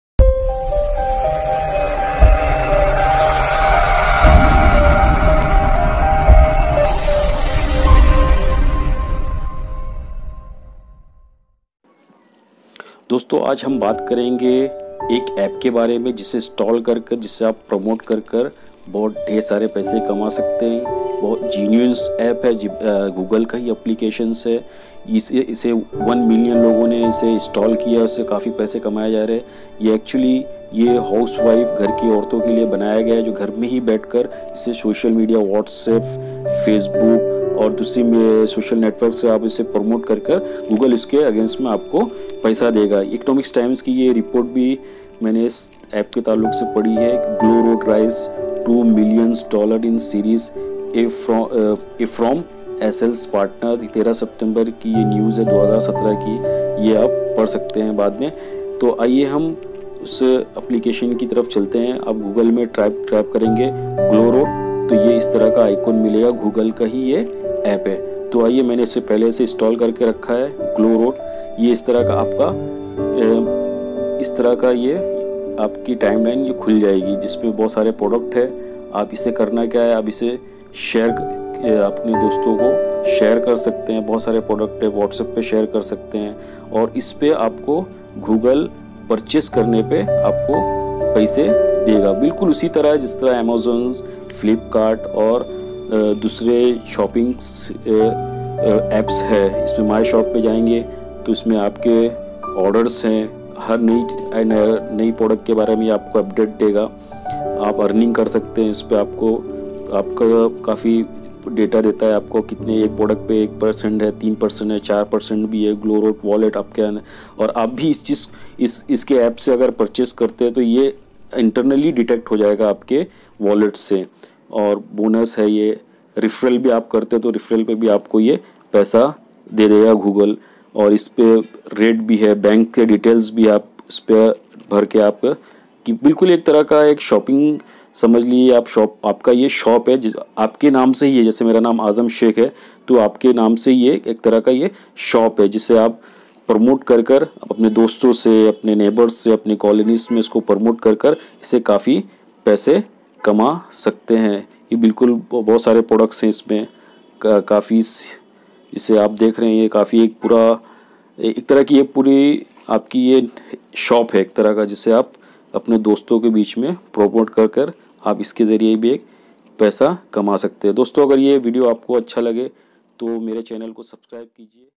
How to Earn Money from Android Apps Glow Road Full Tutorial
Bunch of Knowledge By Azam Shaikh
About Application.
GlowRoad helps women earn from home by reselling a variety of trendy products such as clothing, home furnishing, jewellery etc in their social network. Here you can start a totally flexible home business at zero investment and in a totally secure way.